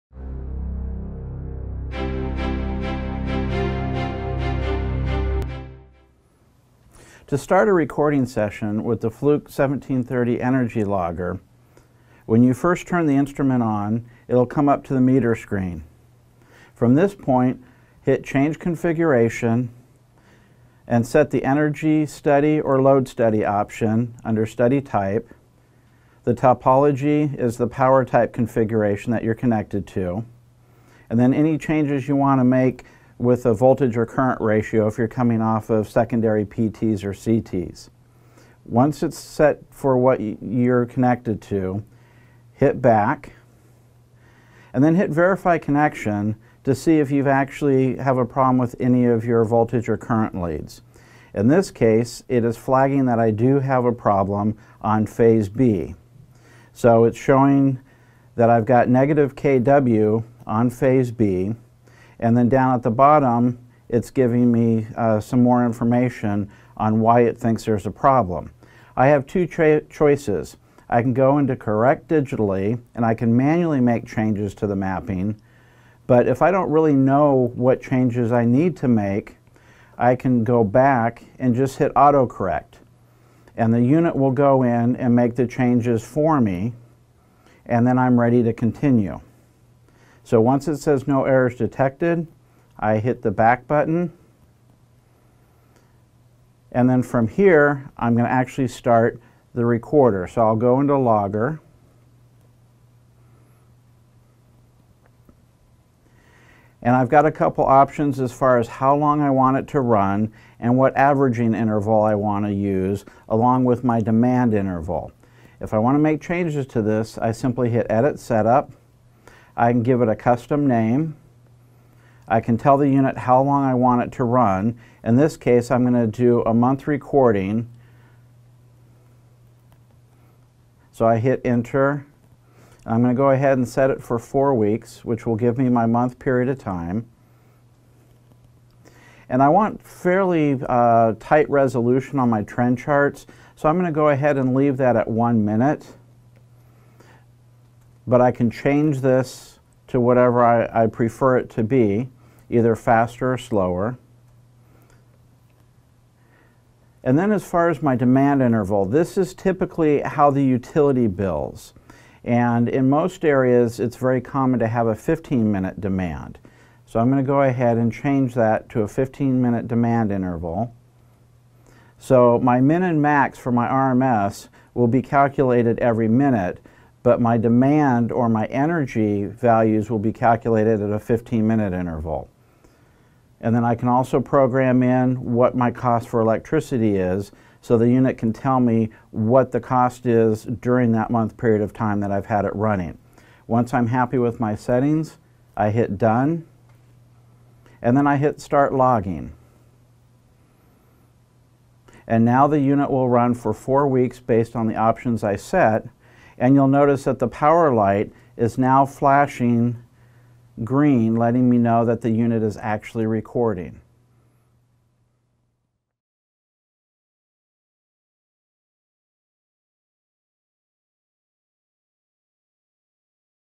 How to Start A Logging Session With The Fluke 1730
In this video, learn how to start a logging session with your Fluke 1730 Energy Logger.

The Fluke 1730 Three-Phase Electrical Energy Logger introduces a new simplicity to discovering sources of electrical energy waste. Discover when and where energy in your facility is being consumed; from the service entrance to individual circuits. Profiling energy usage across your facility helps you identify opportunities for energy savings, and provides you with the data you need to act on them. The new Energy Analyze software package allows you to compare multiple data points over time to build a complete picture of energy usage, which is the first step to reduce the cost of your energy bill.

For more information about Fluke Energy Loggers, visit the Fluke

The Fluke 1730 Energy Logger has been discontinued. Please refer to its upgraded counterpart: Fluke 1732 and 1734 Electrical Energy Loggers.